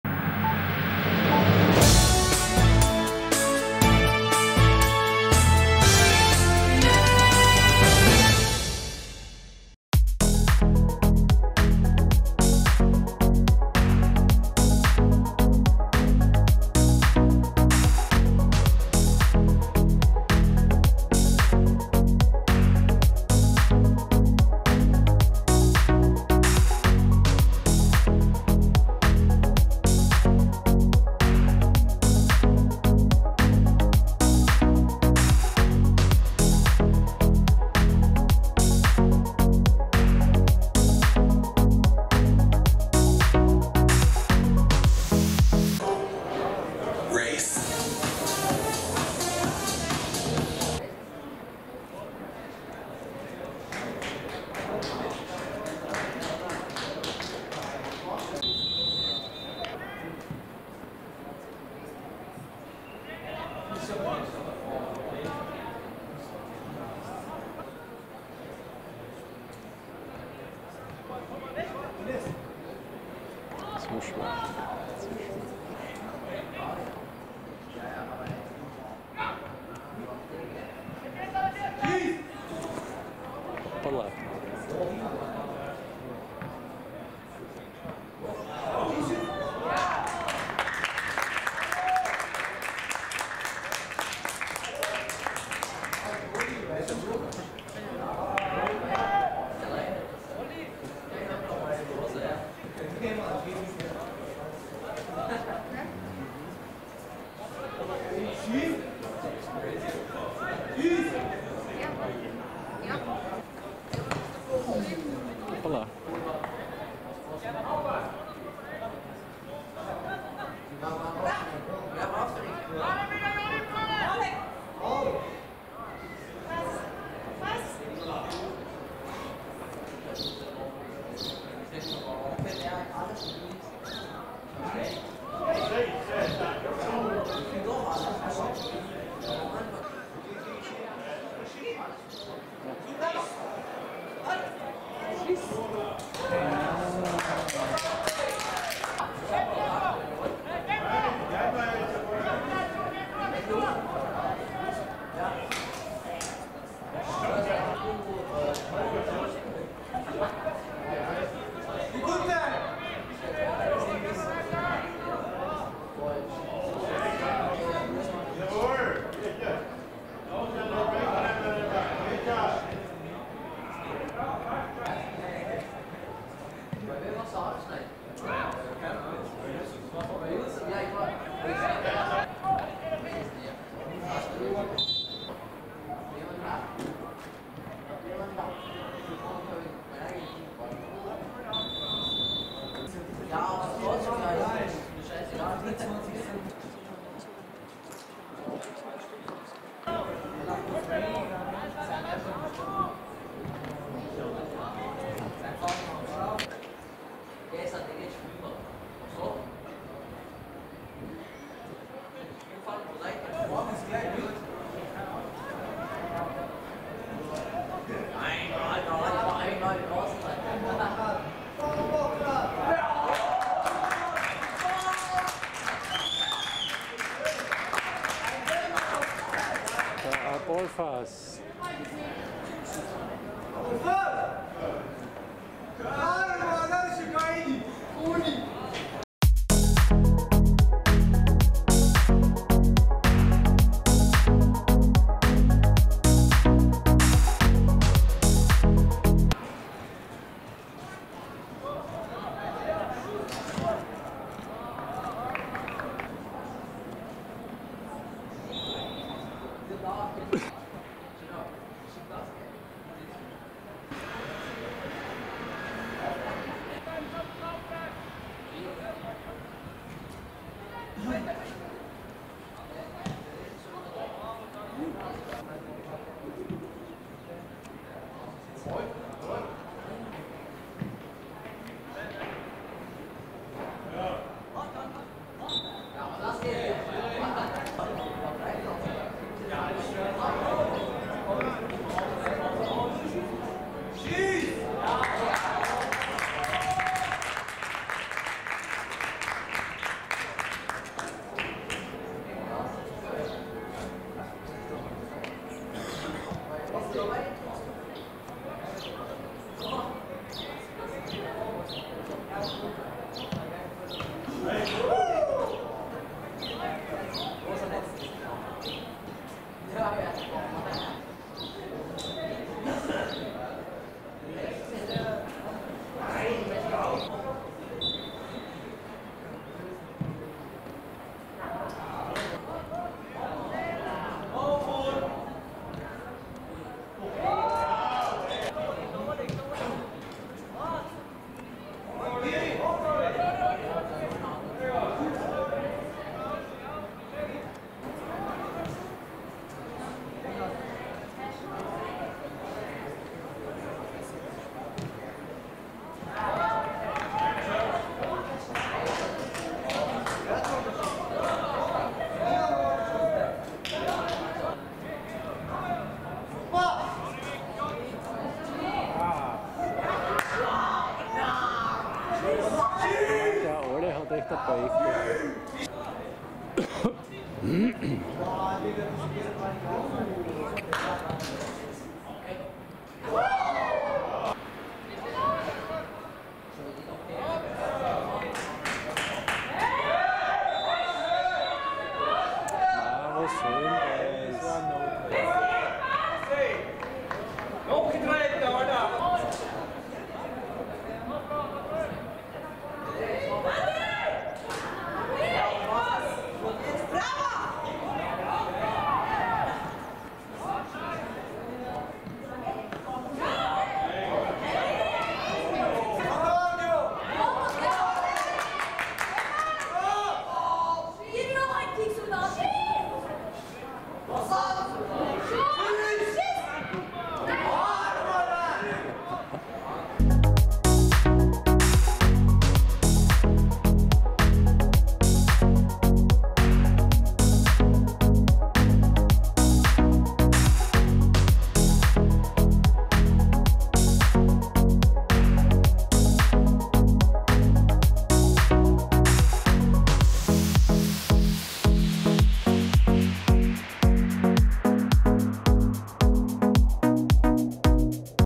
🟣 U16LL ⚽️ ESK Graz 🆚 ABZ Almenland vom 11.5.2024
✅ Highlights :: Landesliga U16 - 7. Runde;
✅ ASKÖ-Stadion Eggenberg;

Unterliga-Schiedsrichter:
Elvedin Beganovic (631 Spiele)